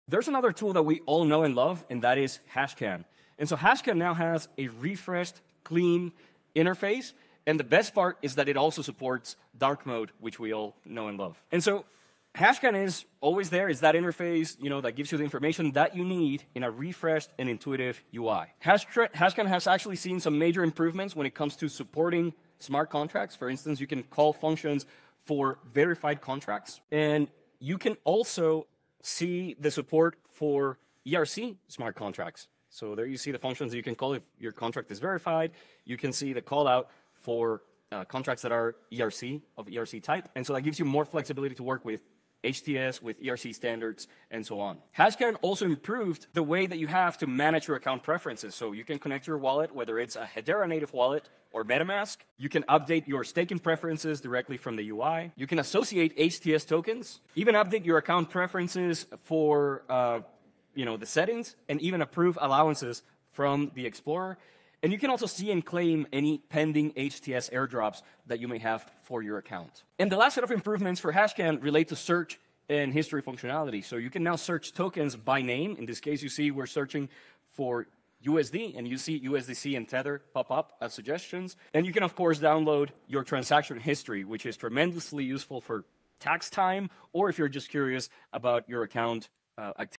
HashScan's New Features: Explore Hedera with an Enhanced UI and Smart Contract Support
Discover the refreshed HashScan explorer with its clean interface and new dark mode! This update brings major improvements for smart contract interactions, allowing you to call functions on verified contracts and providing better support for ERC standards. Connect your Hedera native wallet or MetaMask to manage your account preferences, update staking settings, associate HTS tokens, and claim pending airdrops—all directly from the explorer. The improved search functionality now lets you find tokens by name, and you can download your transaction history for tax purposes or personal tracking. HashScan continues to be the intuitive interface for all your Hedera network exploration needs.